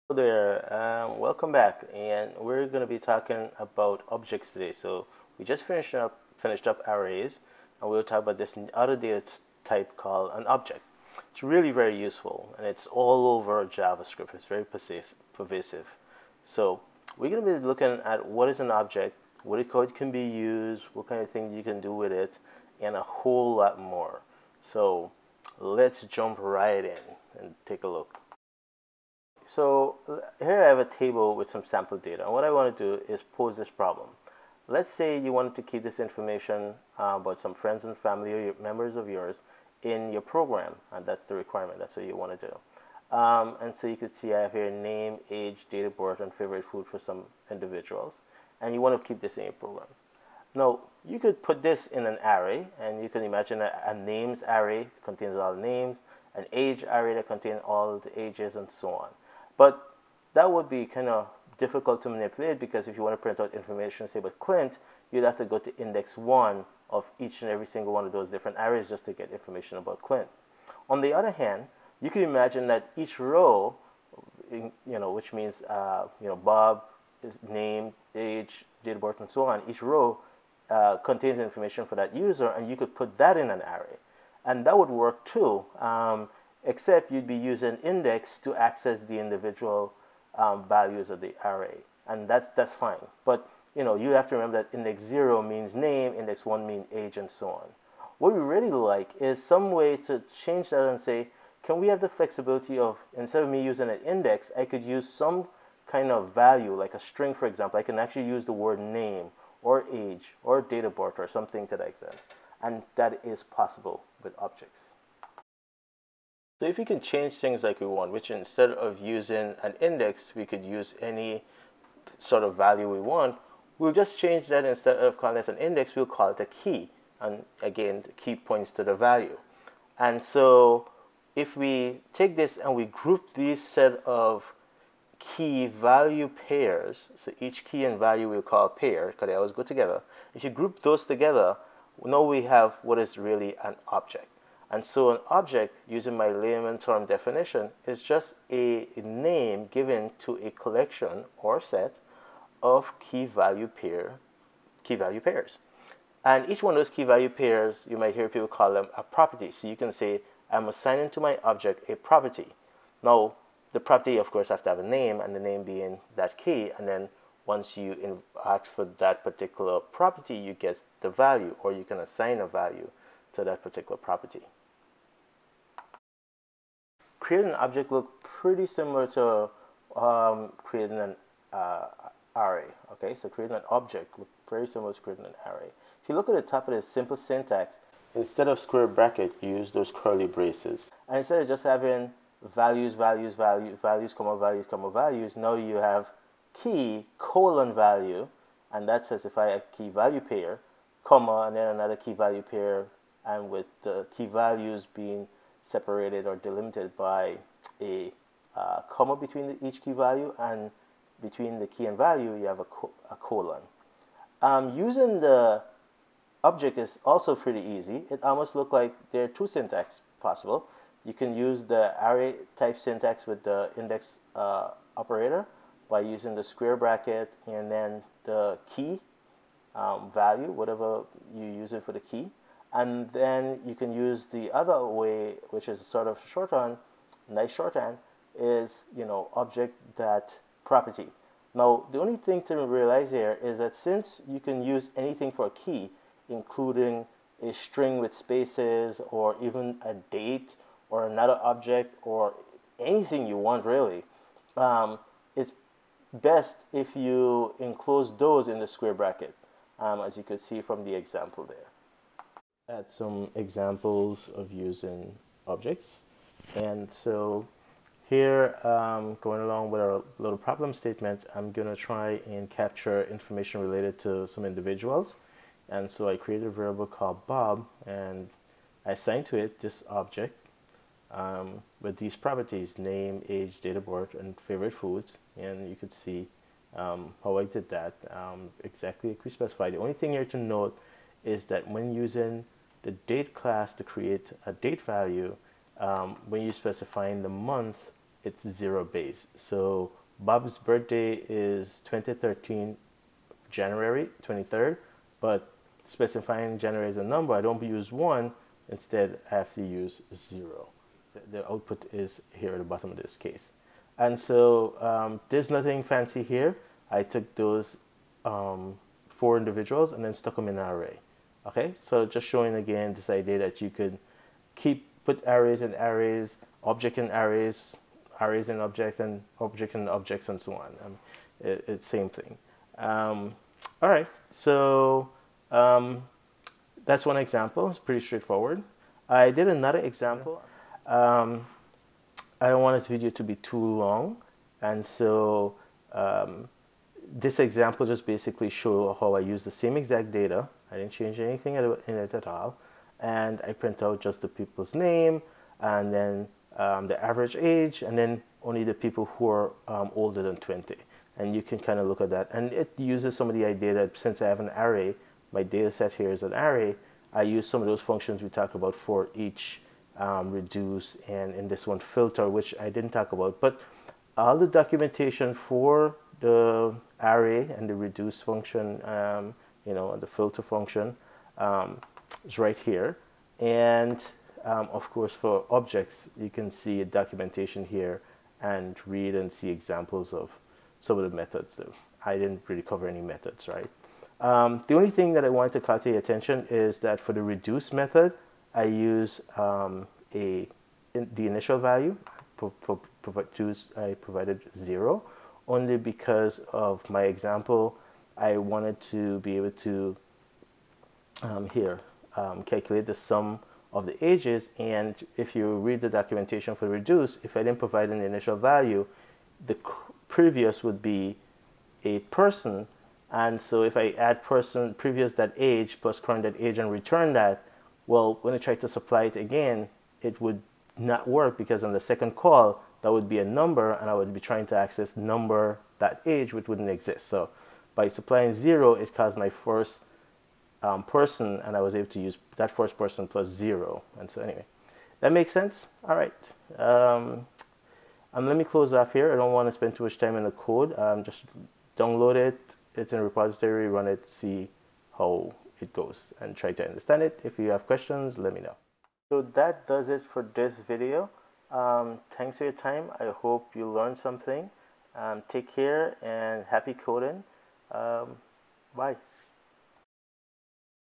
05.14 - #39 - JavaScript - Introduction To Using Object Data Type
Here comes Objects. In this video, we will start looking at Object as a type in JavaScript. The Object type is very flexible and used just about everywhere in JavaScript.

Once you learn about Objects, you will wonder how you ever got anything done without them. :)

So buckle in and let's gets's started.

As usual, the code and slides are in the Git repository You may clone the code or browse it online.